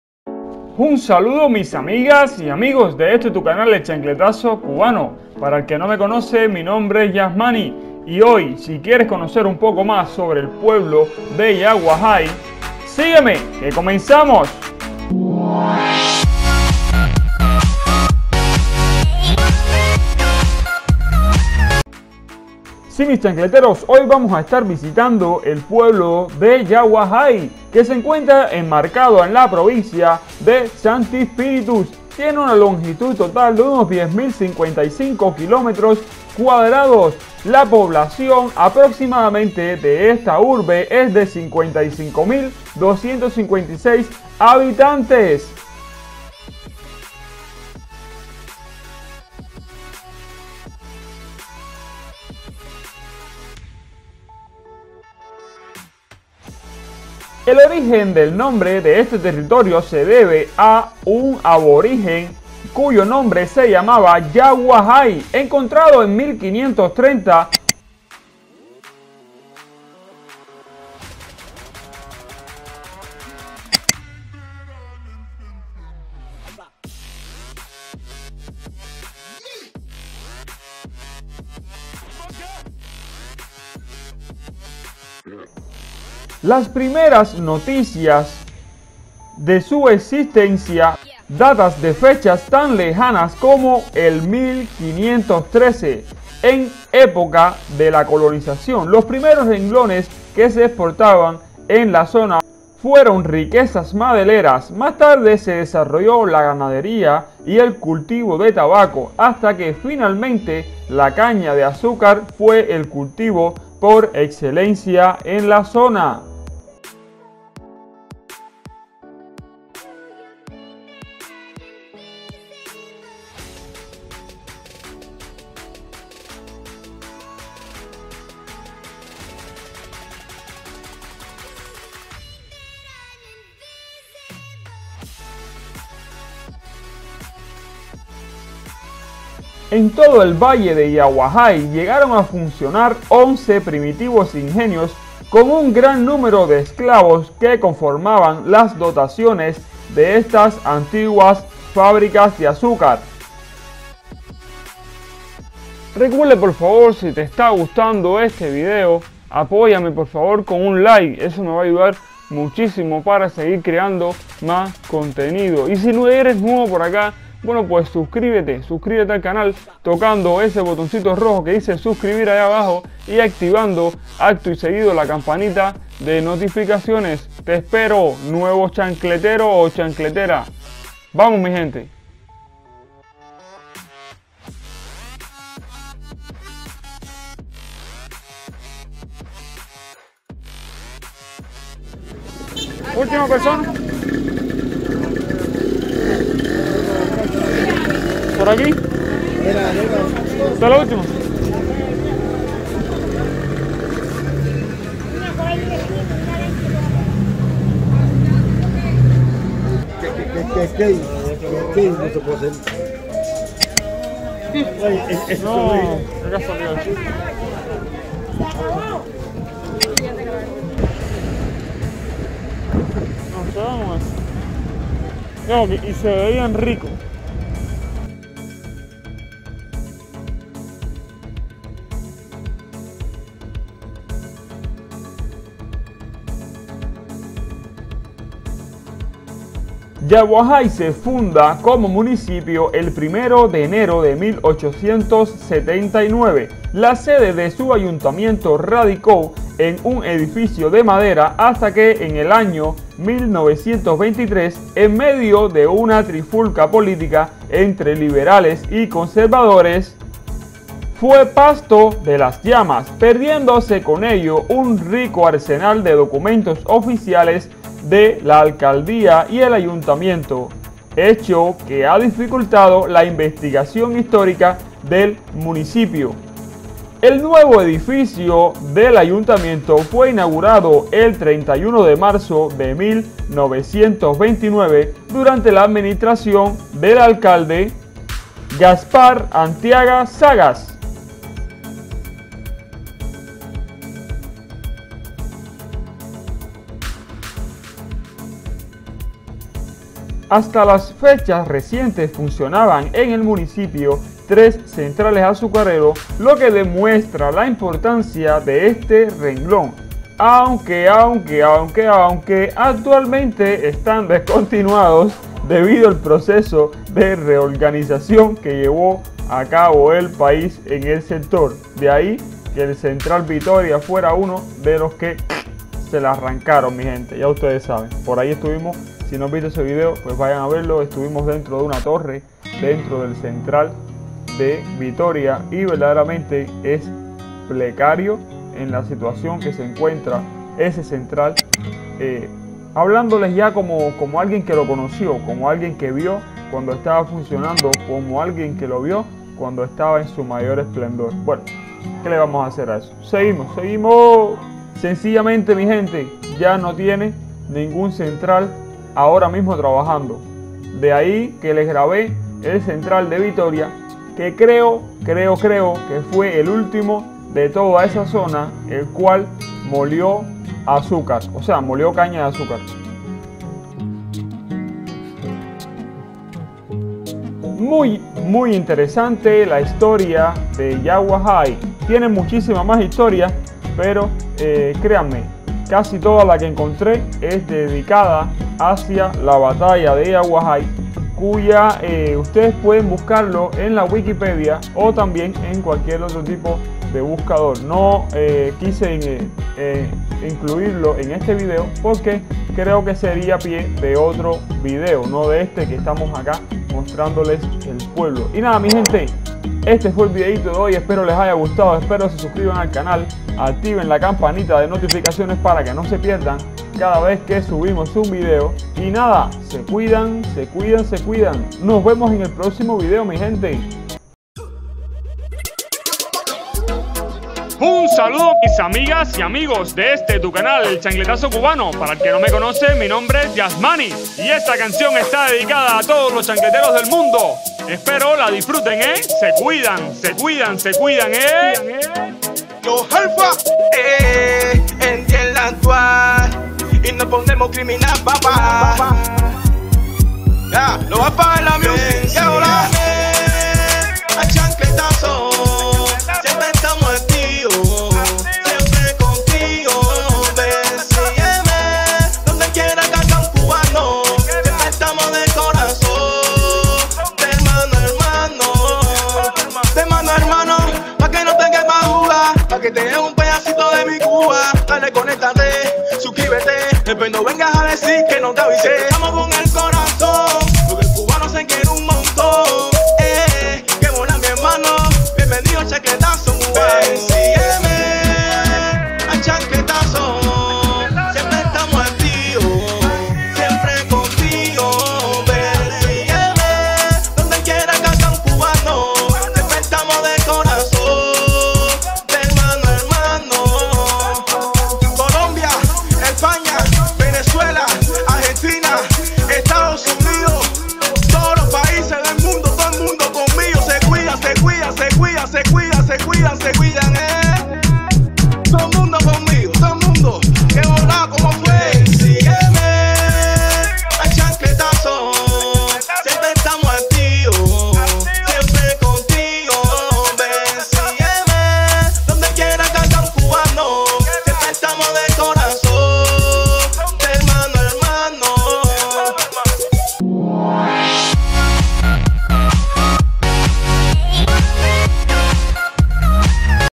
😲 YAGUAJAY un pueblo DEZTRUIDO pero con una HSTORIA increíble vuelta a CUBA en moto
Saludos mis chancleteroz hoy recorremos el pueblo de YAGUAJAY les muestro algo de su historia espero les guste se cuidan eeeeeeeee